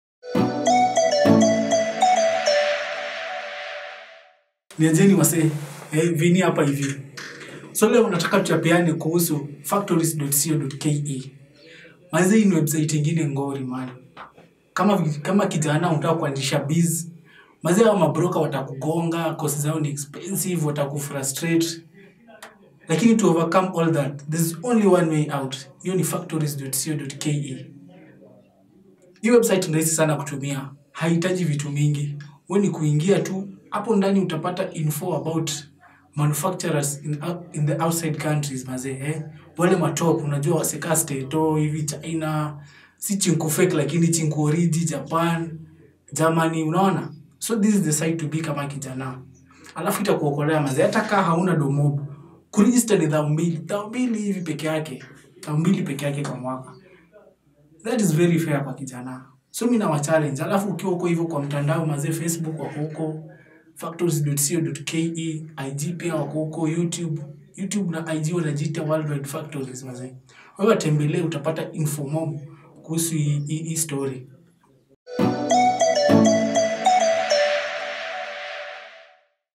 Form za bizna
Niaje wasanii io vida inakuchapia kuhusu factories.co.ke.
Ni website moja ngori sana...imagine iko na info kuhusu manufactures 800k all over the world.
Maze mtembelee io website ..ni ya maana sana